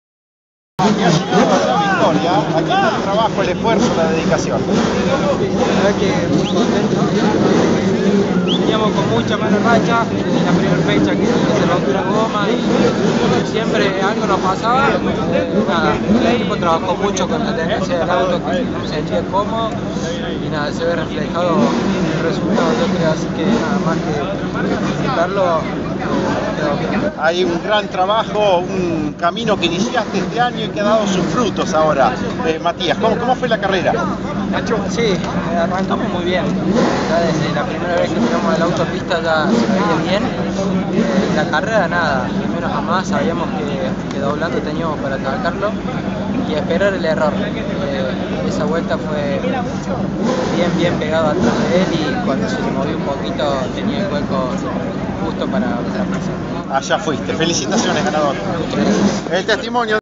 Cravero alcanzó su primera victoria en el TC Mouras
El piloto de Dodge se adjudicó la novena fecha del TCM después de largar segundo en La Plata y superar a De Bonis a 3 vueltas del final.